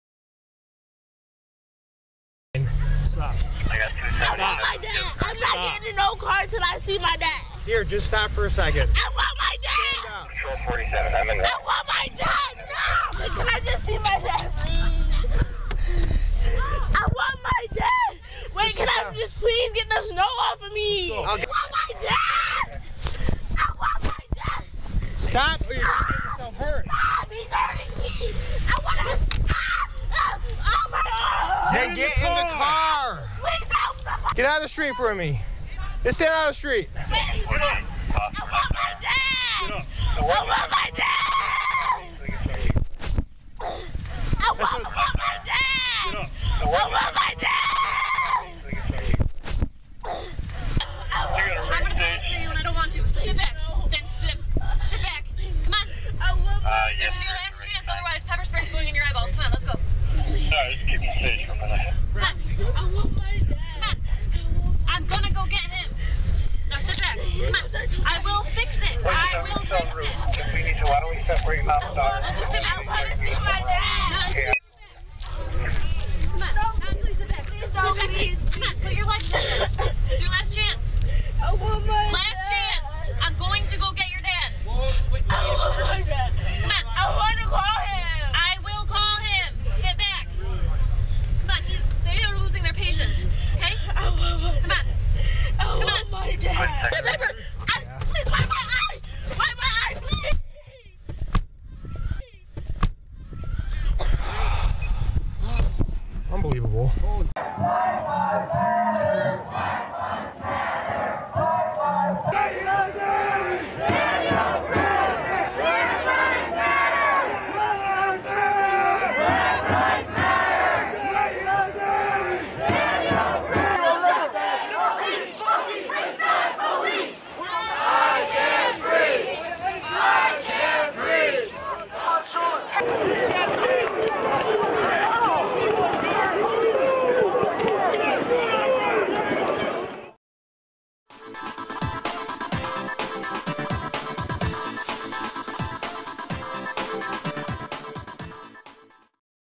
Body-cam shows 9-year-old pepper-sprayed
Police officers in Rochester, New York, are under investigation again.
This, after a camera footage showed officers handcuffing and pepper-spraying a 9-year-old girl.

Officers were responding to a family disturbance call on Friday

Video shows police wrestling the girl to the ground in the snow